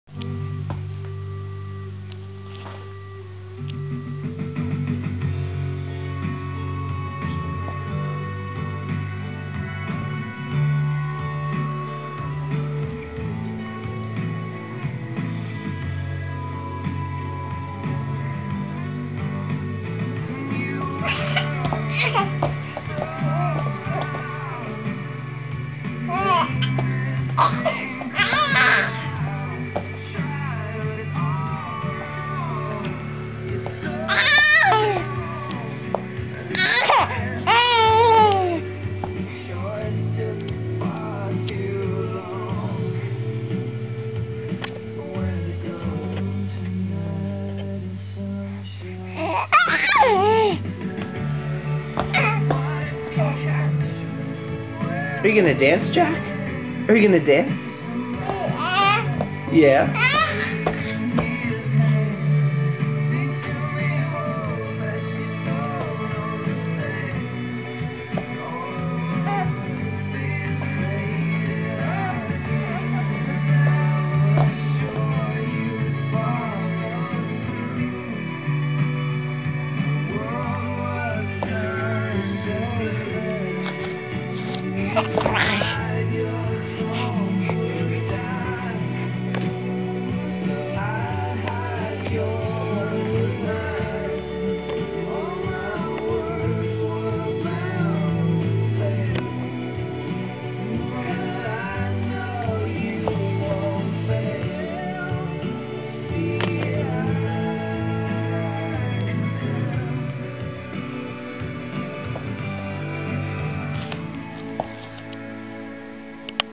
Jack Reach Out and Touch Someone
Between crazy moves, Jack echoes AT&T commercials and tries to reach out and touch someone